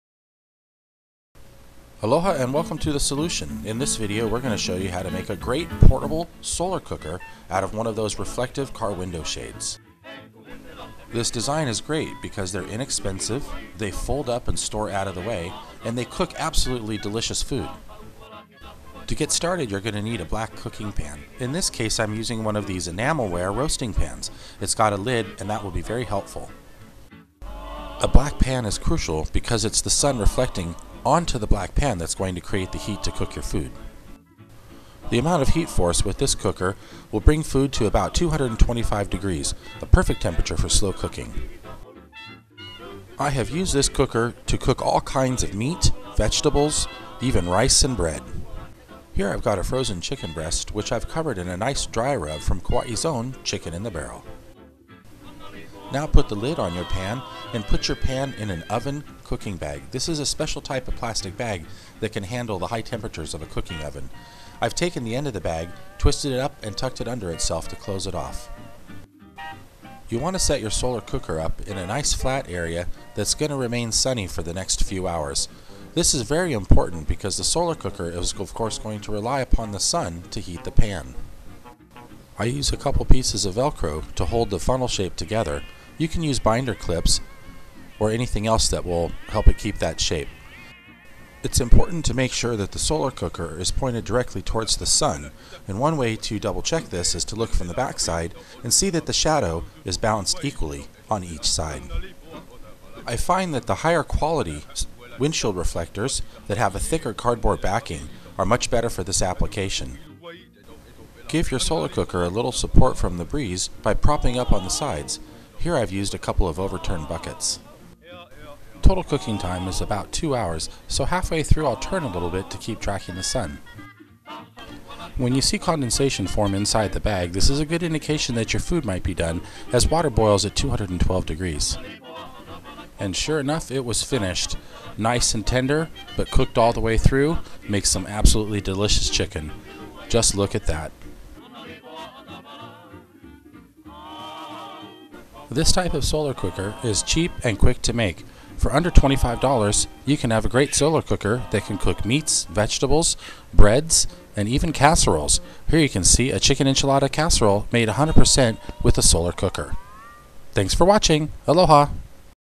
Making a great Solar Cooker from a Car Windshield Sun Shade that actually works!
You can make this portable solar cooker for under $30.00 and it really works great for cooking meats, vegetables, breads, rice, casseroles and much more. With cooking temps running around 225°F+ your meats will come out tender and fall right off the bone. Your veggies will be cooked perfectly, all the way through but with still the perfect amount of crunch left! Can be used when the sun is low in the sky and when it is right overhead.  If you can see your shadow you can cook. No gas, charcoal, electricity needed... just the sun!